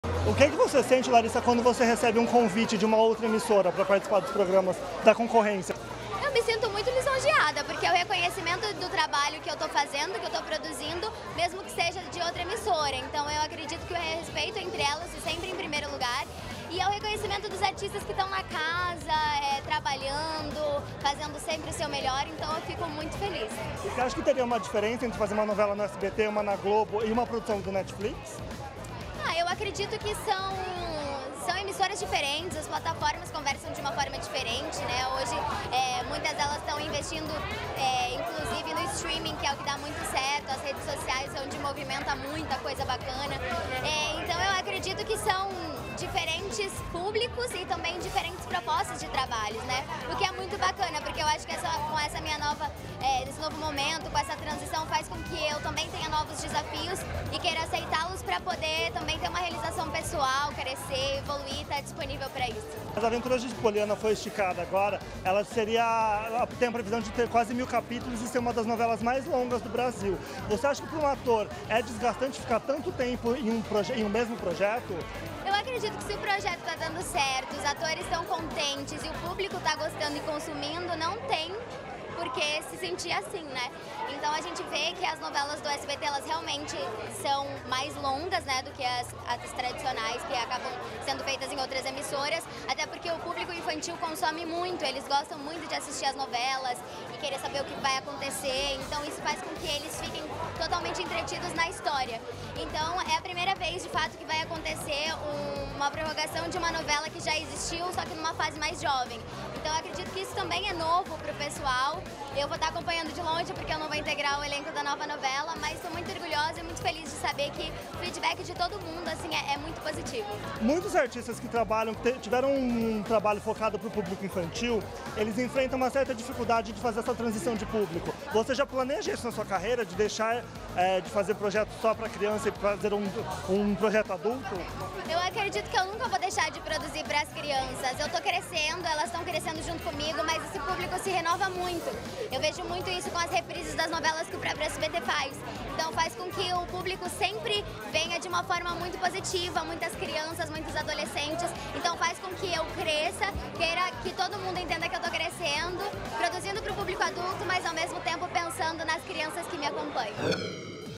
Larissa Manoela diz que não vê problema em novela "esticada" por emissora
Larissa Manoela vai deixa o elenco de “As Aventuras de Poliana” no final do ano, a novela, no entanto, deve continuar até 2021, se tornando uma das novelas mais longas da TV brasileira, com mais de mil episódios.

A atriz comentou sobre esse “esticamento” promovido pelo SBT. "Se o projeto está dando certo, se os atores estão contentes e se o público está gostando e consumindo eu não vejo problema", destacou.

“As novelas do SBT realmente são mais longas (…) até porque o público infantil consome muito (…) eu vou estar acompanhando de longe, até porque não estou no novo elenco da novela”, completou a atriz.